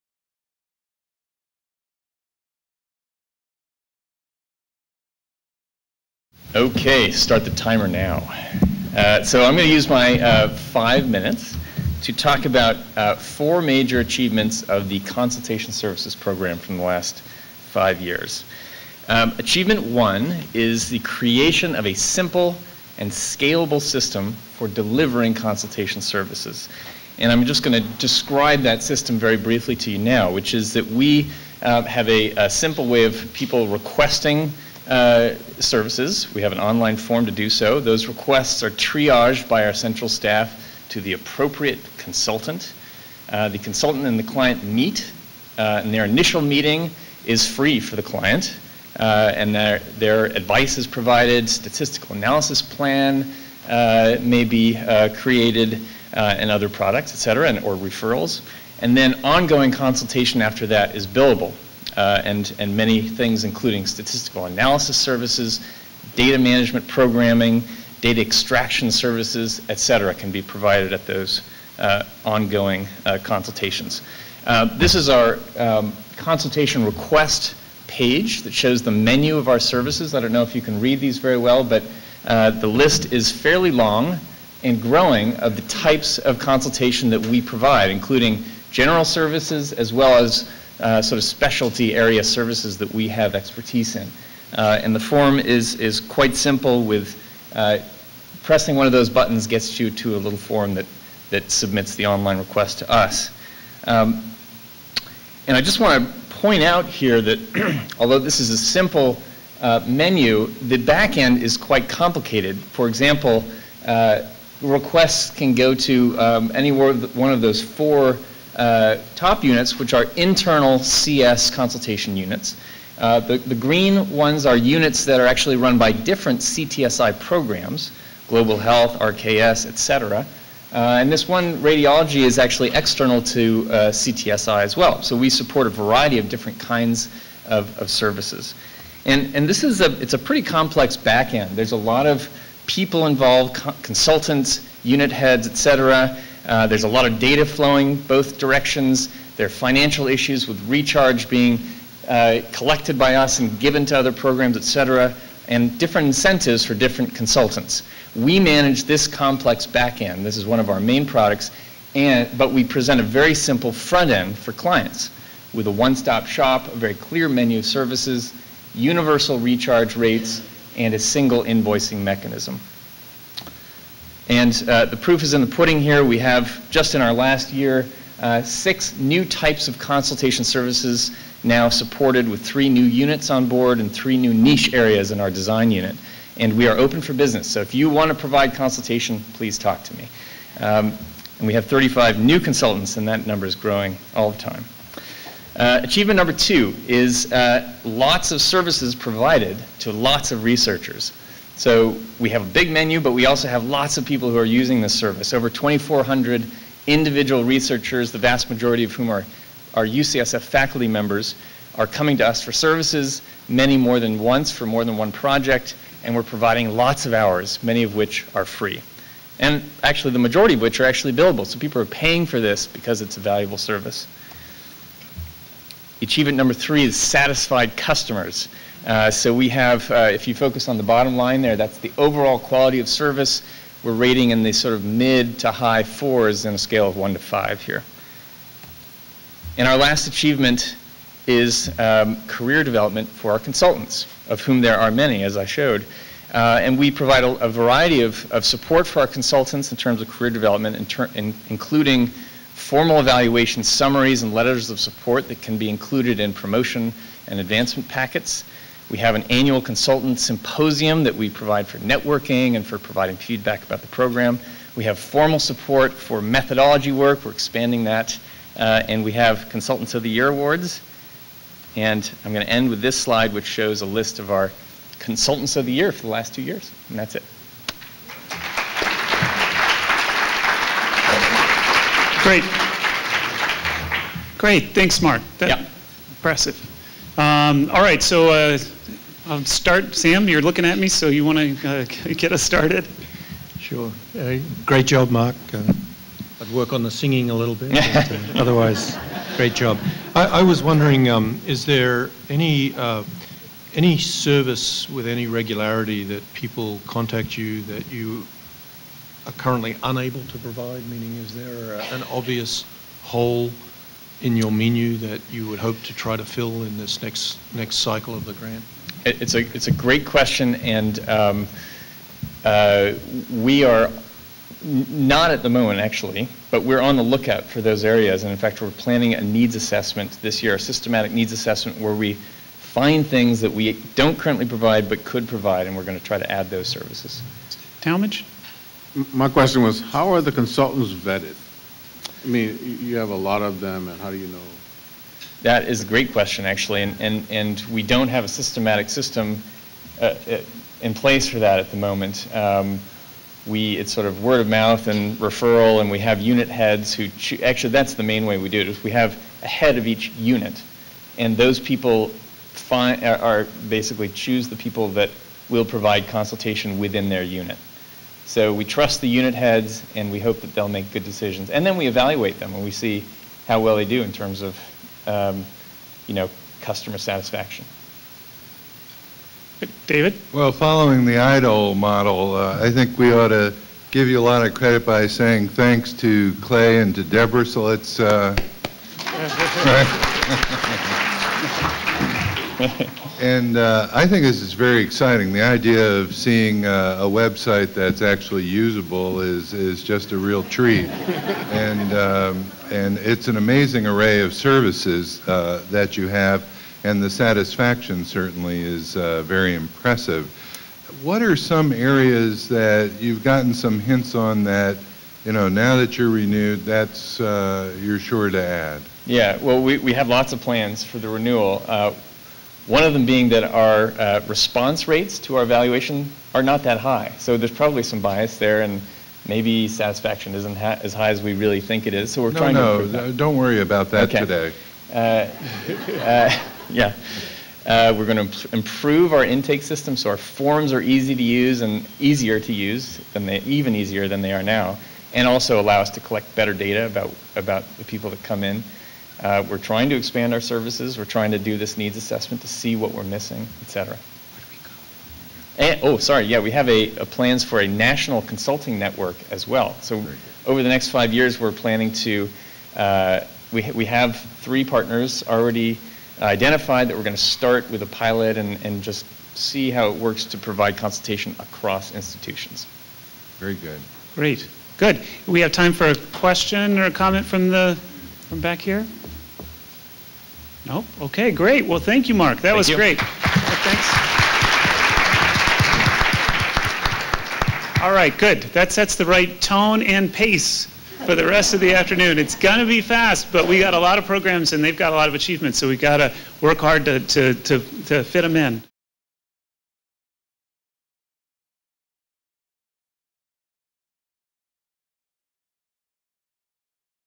Mark Pletcher talks about CTSI Consultation Services: CTSI Annual Retreat 2011 - Part 2
Among the highlights of the retreat was an American Idol-style "competition" that showcased presentations from CTSI programs, with each presenting examples of key achievements and measurable progress during the past five years. Playing the role of "judges" were Talmadge King, MD, chair of the Department of Medicine at UCSF; Sam Hawgood, MBBS, dean of the UCSF School Medicine; and David Vlahov, RN, PhD, Dean of the UCSF School of Nursing.